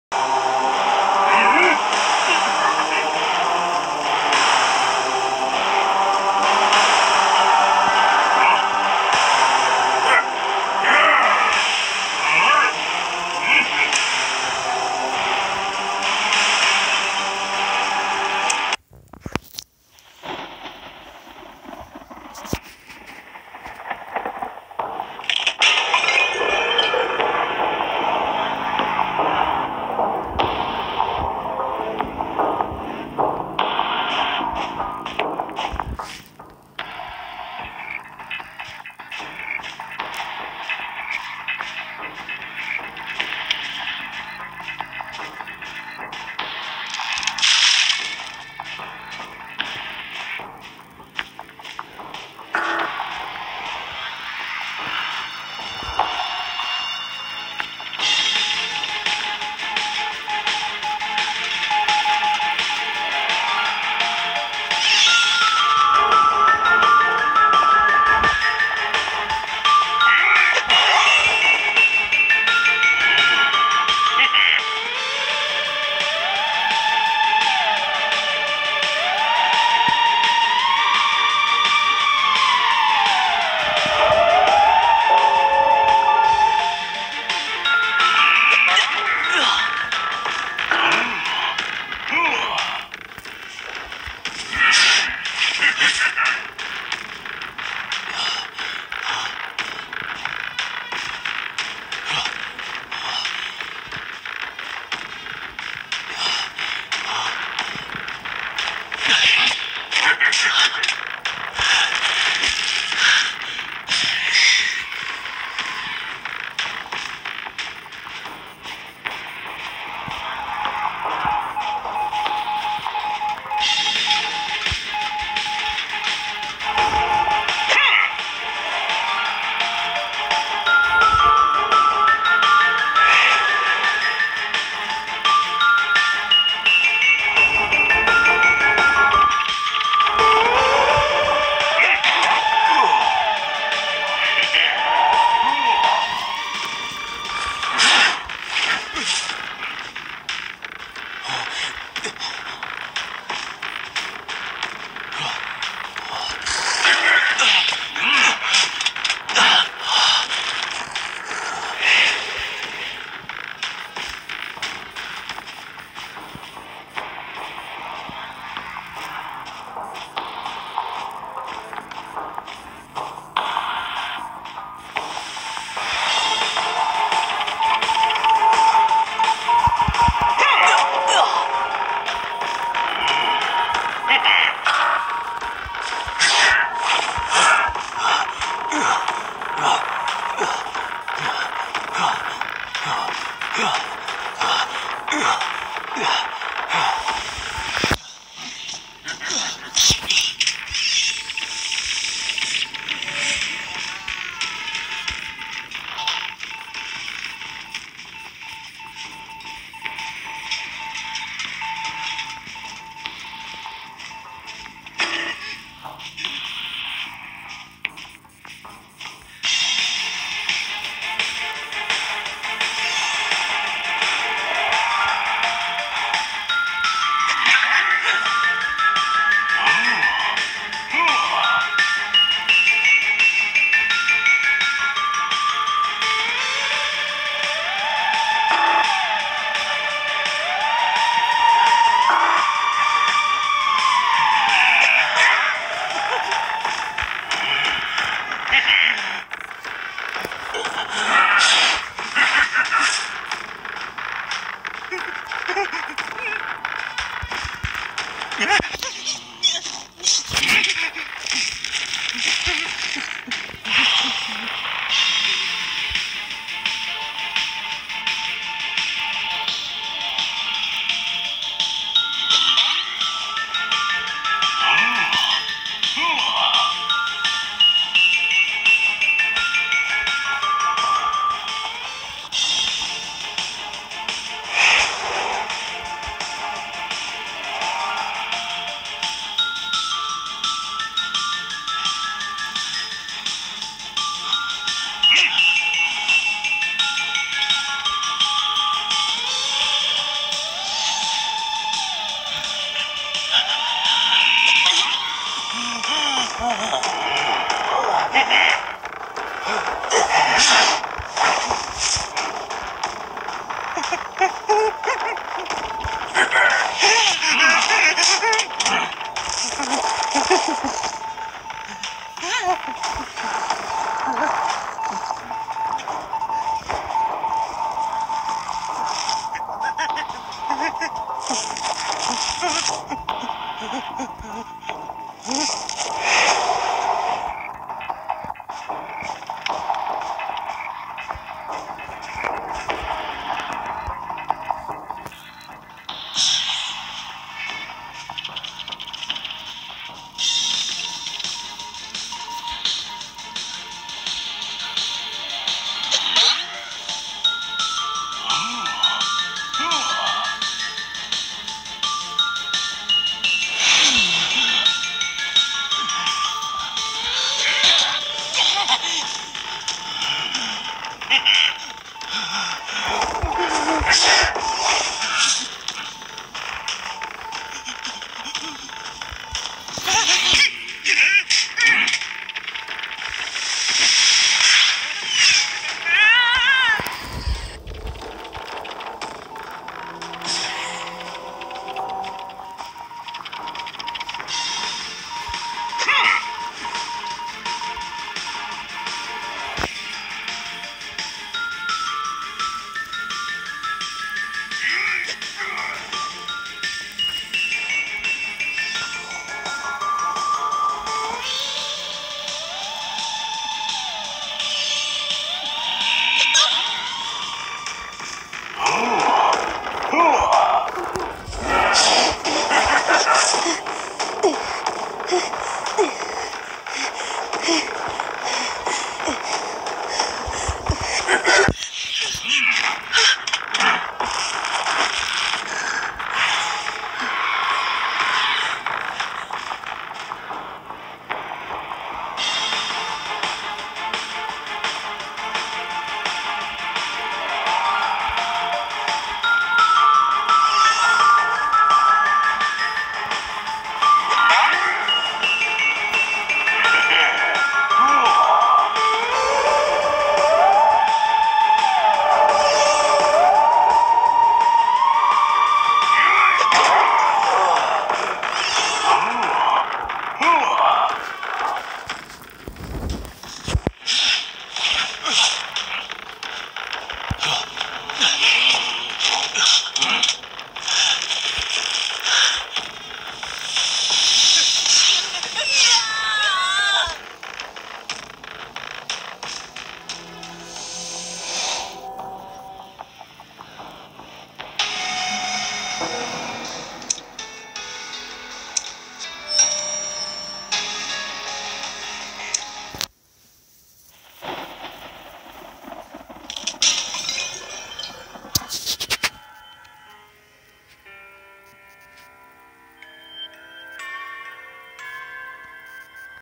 Showing off the Aladdin skin for Hell Ember
In this video, I show a match with bots (because I have not used Hell ember in a long time) to show off the halloween skin for the hell ember based on the Magician's Blue Aladdin skin. I also show off the red church map halloween version and show off the halloween music.